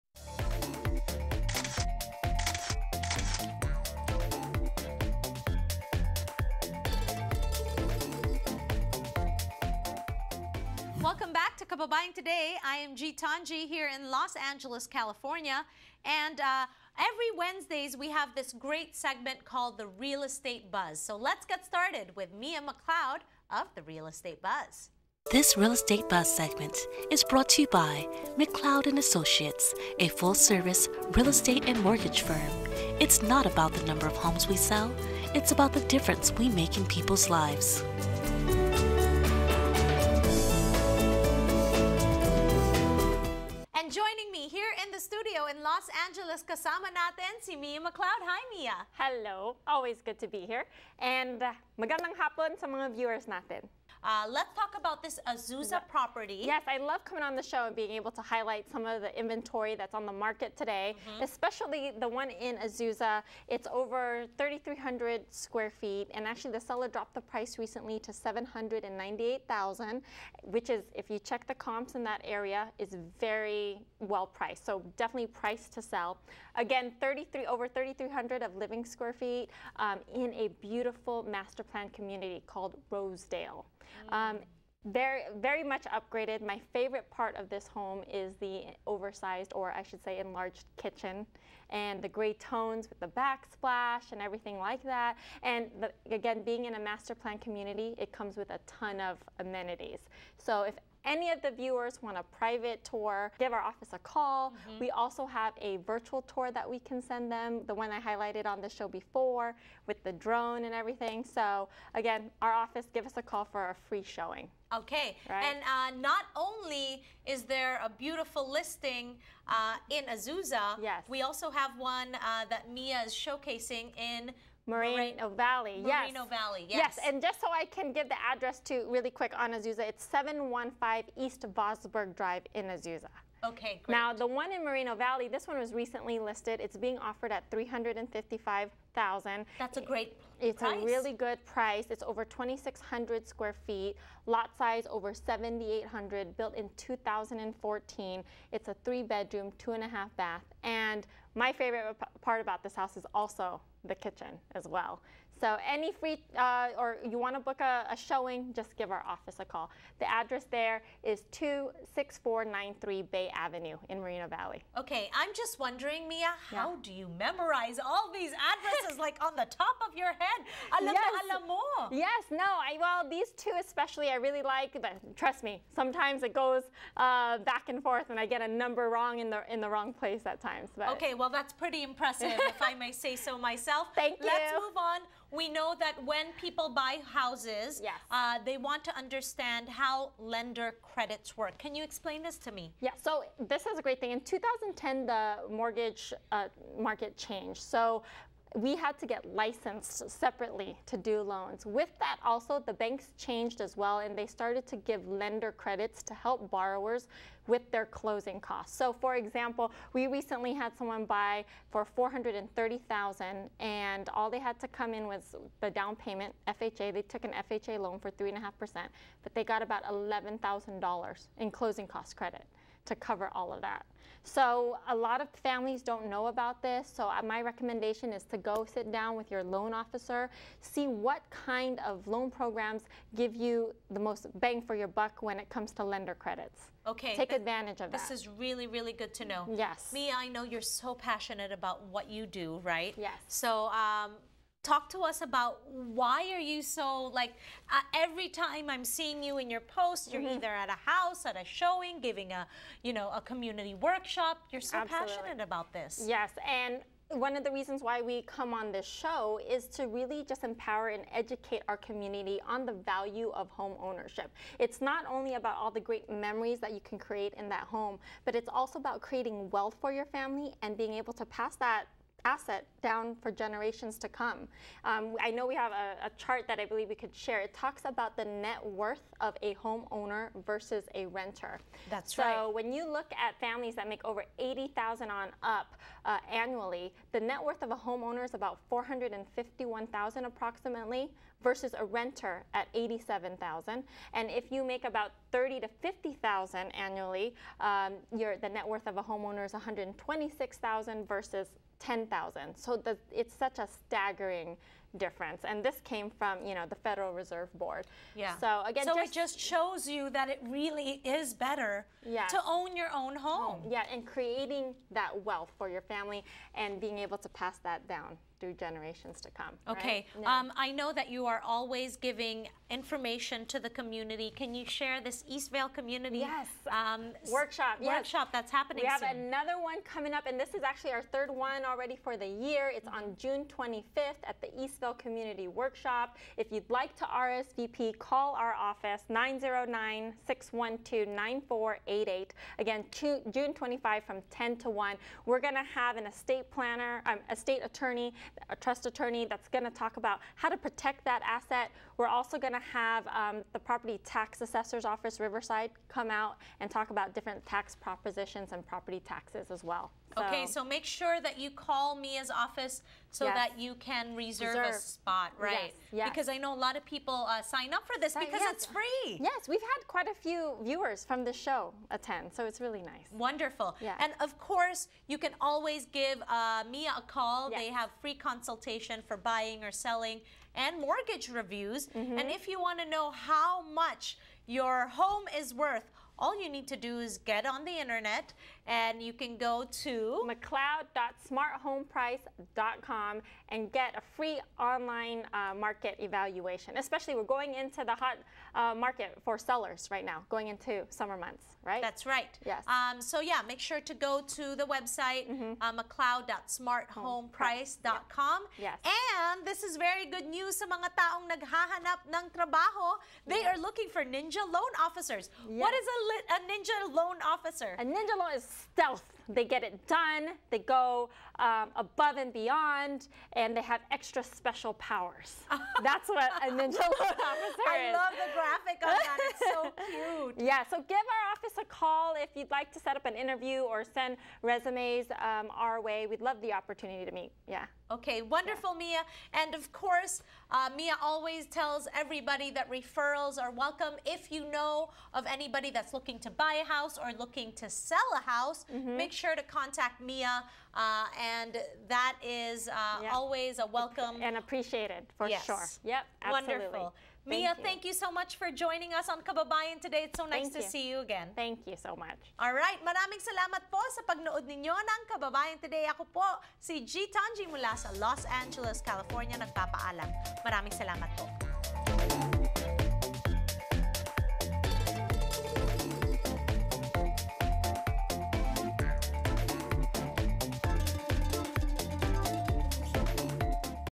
Mia Mcleod Shares Available Listings & How Lender Credits Work
Original Airdate Published on May 18, 2016 Part 4 of 4

Mia Mcleod, Real Estate Expert shares listings of Azusa & Moreno Valley Listings. Mia also educated Kababayans on how lender credits work. Watch for details on her upcoming EastVale Community Workshop this June 2016.